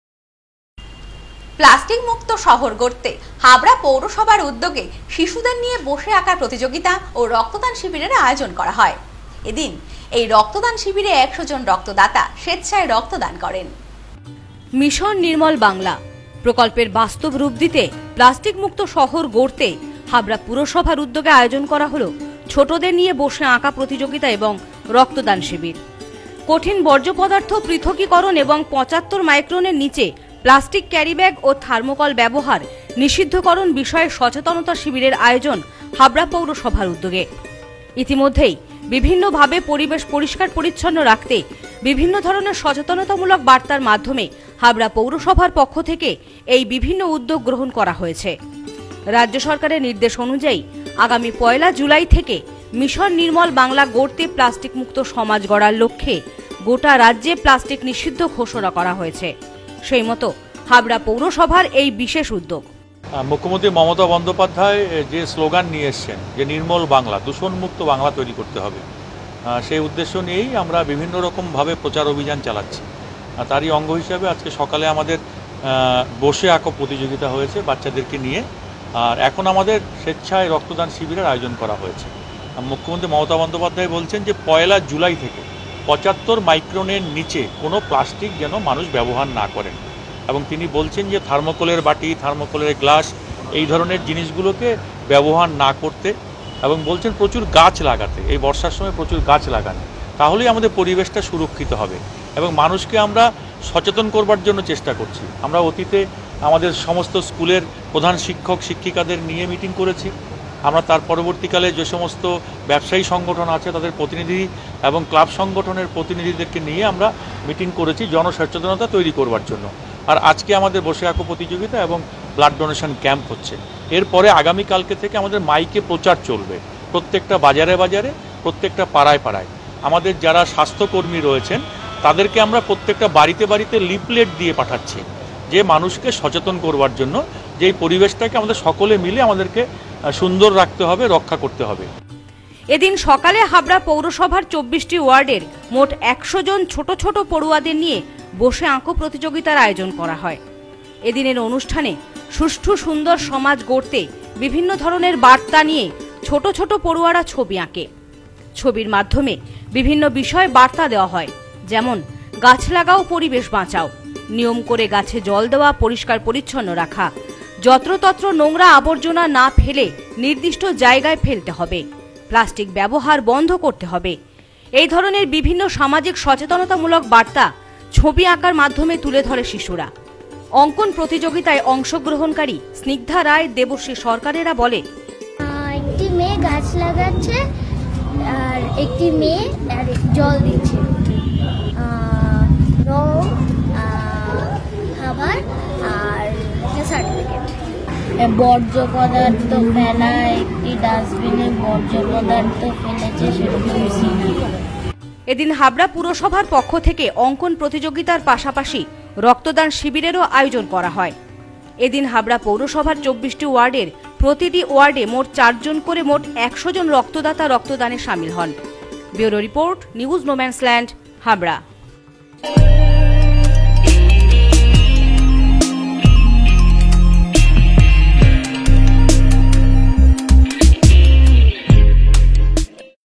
প্লাস্টিক মুক্ত শহর গড়তে হাবড়া পৌরসভার উদ্যোগে বসে আঁকো ও রক্তদান শিবির
প্লাস্টিক মুক্ত শহর গড়তে হাবড়া পৌরসভার উদ্যোগে শিশুদের নিয়ে বসে আঁকো প্রতিযোগিতা ও রক্তদান শিবিরের আয়োজন। এদিনের শিবিরে প্রায় ১০০ জন রক্তদাতা স্বেচ্ছায় রক্ত দান করেন।